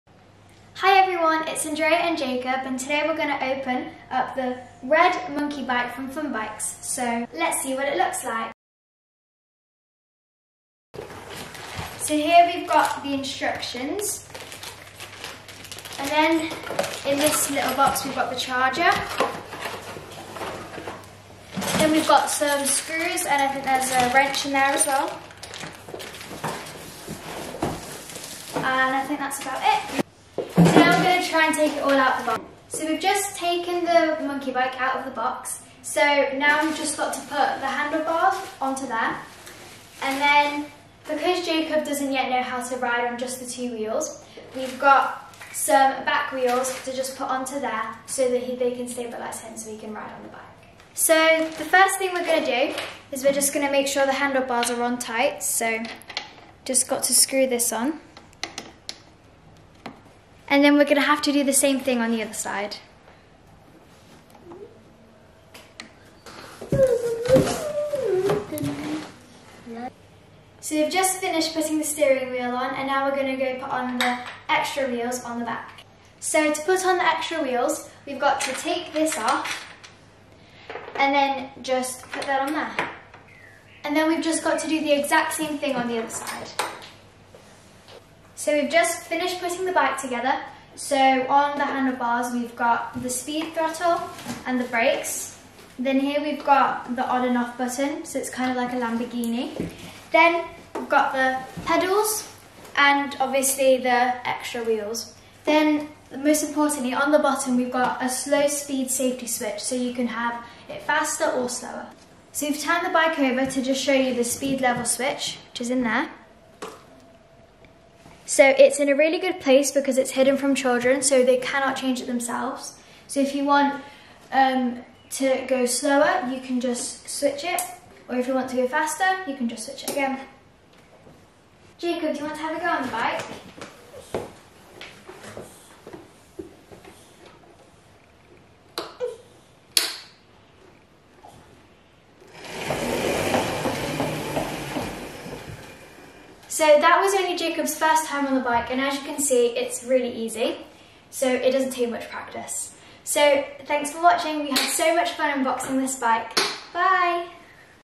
Unboxing The FunBikes MB 43cm Motorbike 250w Electric Kids Monkey Bike
FOR MORE INFO PLEASE VISIT FUNBIKES.CO.UK

FunBikes MB 43cm 250w Electric Kids Monkey Bike

"New Models now come complete with complimentary stabilisers and knobbly tyres"

Just like the name suggests, this new bike takes things to a new cheeky fun level for the junior rider and has to be the ultimate kid’s e-bike! But, don’t just take our word for it, take a look at these awesome features: powered by a large 250-watt motor, with a high and low-speed setting, the highest can reach speeds of up to 20kph very quietly, therefore, no noise, no pollution! This beauty also comes with 24volt batteries giving the mini Funbike a smooth ride – awesome for some outdoor fun!

This is a great bike, check out its twist throttle and rear disc brake giving it a very responsive acceleration, just switch it on and go!

We know that you want to look awesome on this bike as if you are riding a real motorbike bike which is why this bike comes with a large chopper-style padded seat and alloy wheels.

Overall Length 97cm, Width 59cm & Height 67cm. A seat height of 44cm. 65kg max rider weight and a top speed of 13mph based on weight and terrain.

Just for reference, we find this is most commonly purchased for children aged 3-6 years. The suitability of the product to a particular child is at the Parents Discretion – Height, Weight & Skill should also be considered.